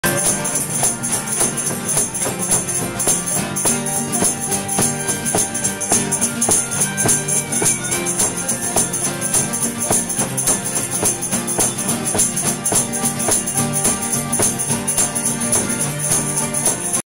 Marina Irvine Ayrshire Scotland
Tom on the Bodhran jamming with freinds at the Folk Club Marina Ayrshire Scotland